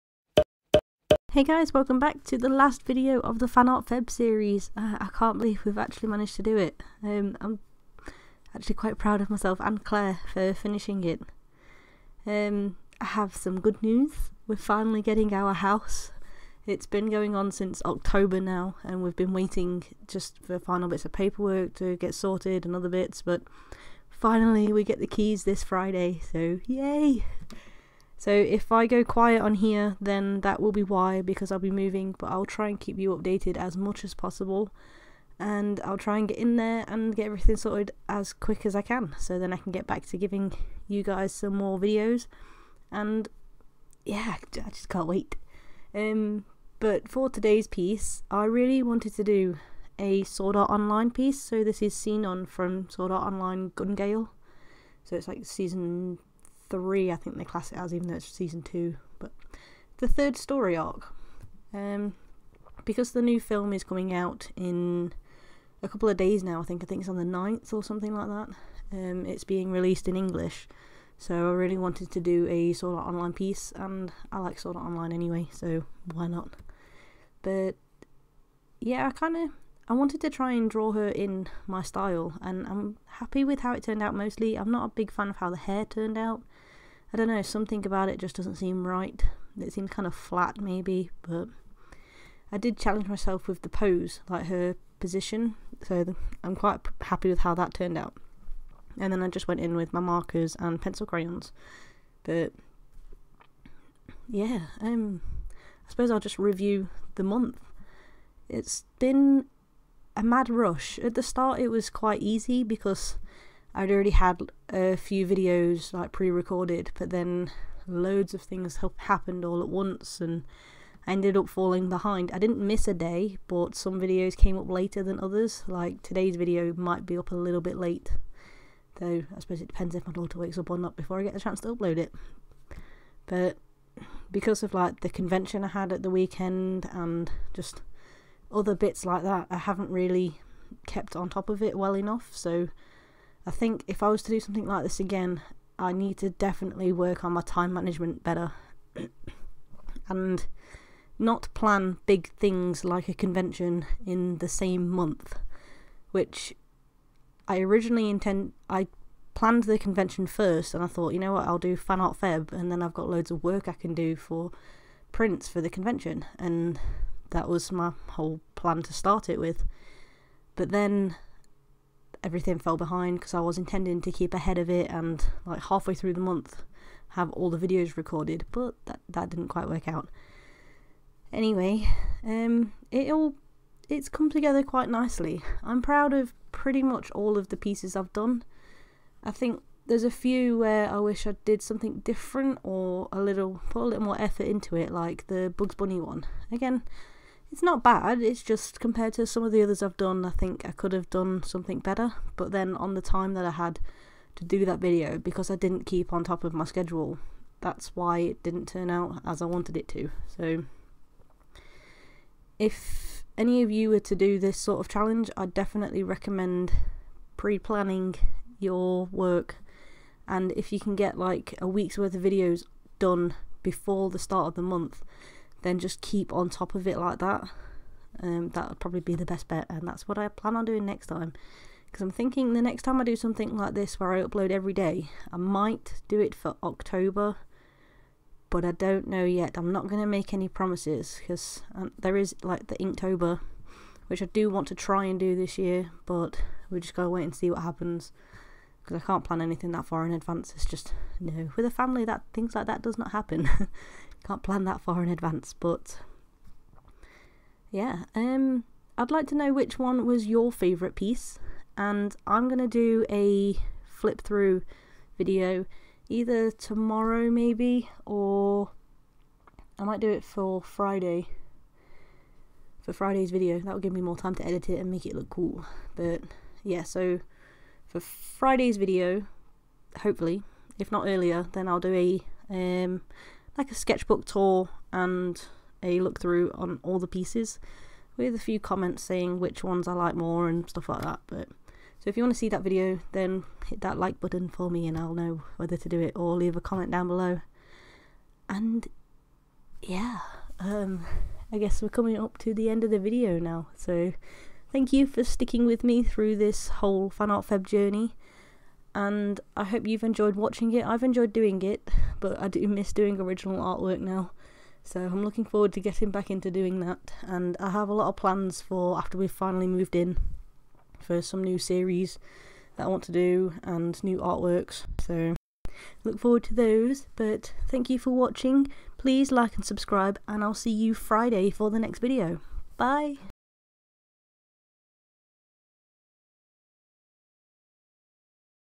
FANARTFEB DAY 28 - SAO - SINON
We've finally made it to the final part of fanartfeb 2017! Thank you for joining me throughout the month!
For todays video i'm drawing Sinon from Sword Art Online.